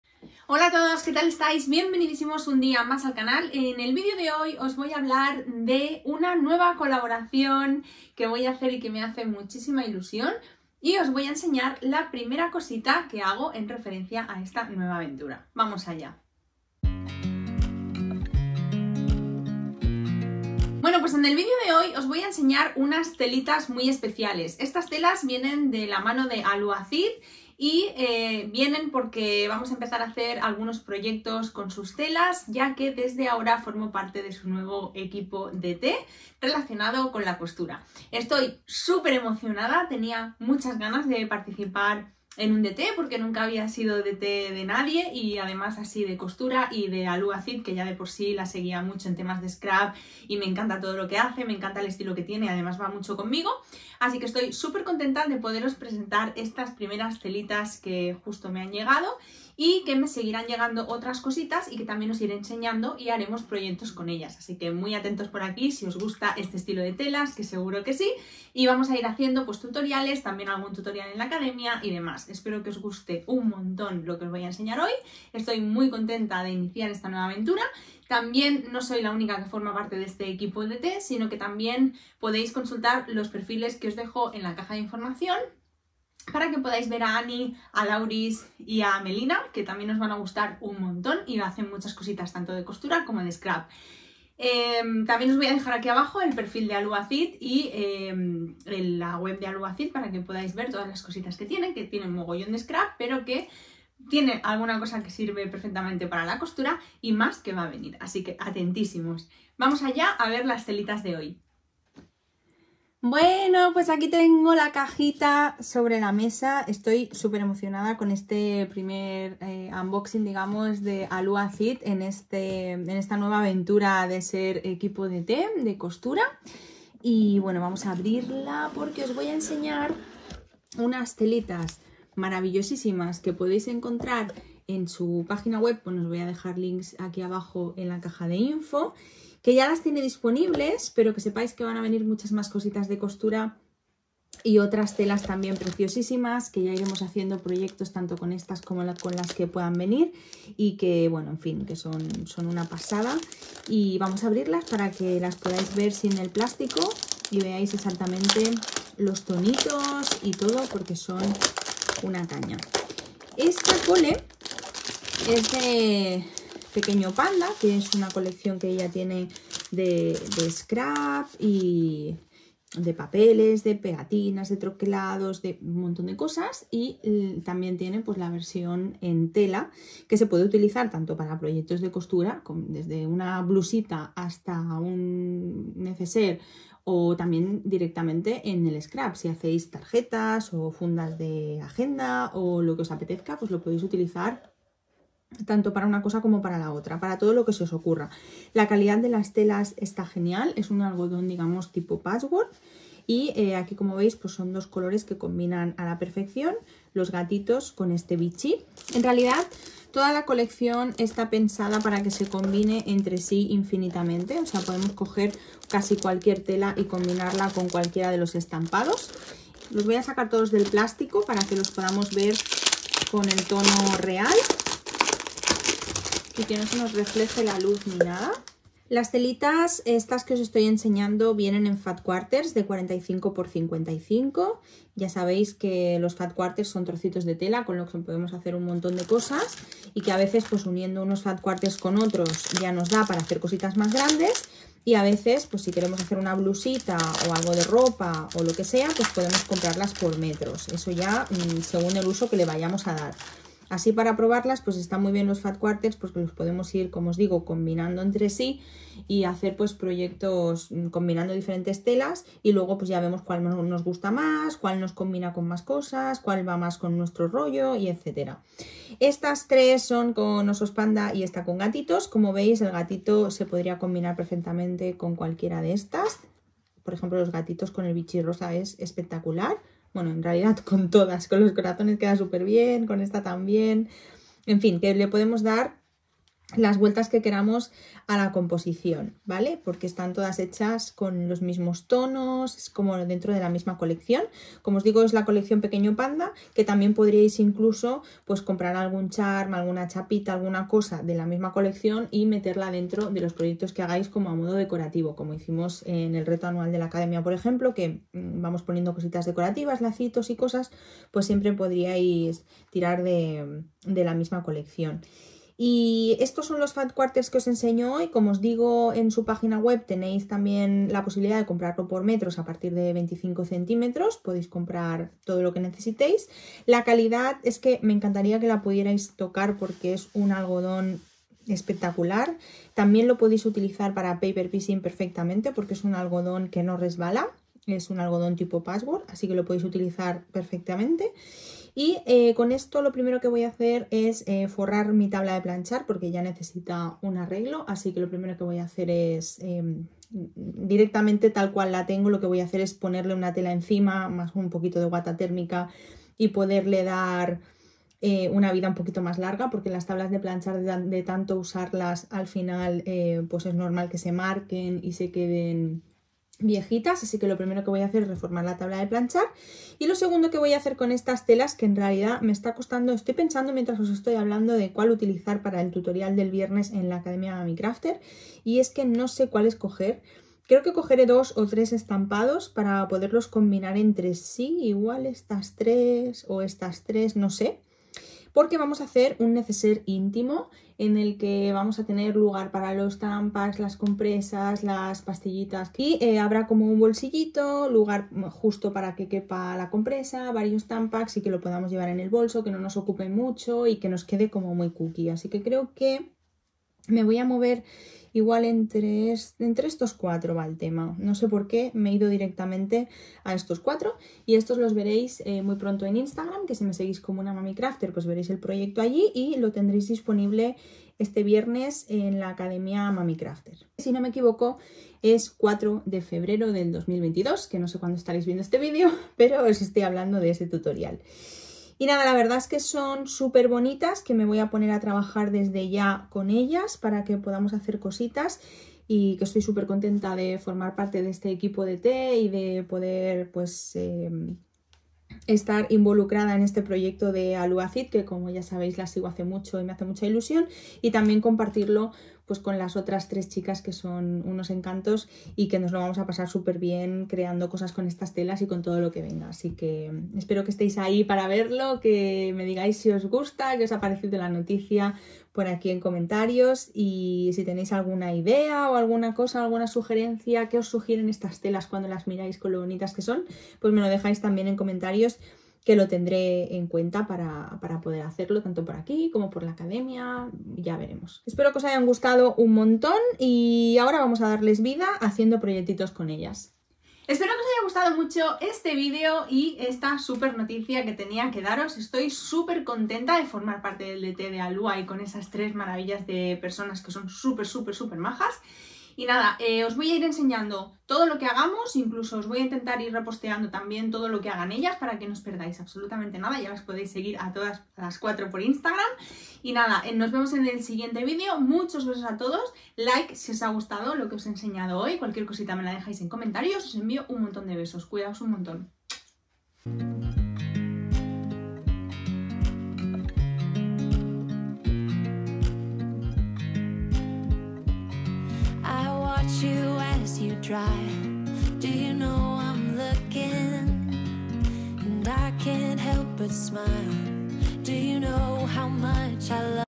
Haul telas Alúa Cid DT Costura
🎬En el video de hoy vamos a ver unas telitas de Alúa Cid y os voy a explicar una noticia que me hace especial ilu😍😍

Voy a formar parte del equipo DTCOSTURA de Alúa Cid junto a Melina de @azulimenta, Ani de @scrapycosturillas y Lauris de @mislulitas_scrapera.

Materiales que uso en mis tutoriales y os pueden interesar, en mi LISTA DE AMAZON:

💋BESOSSSS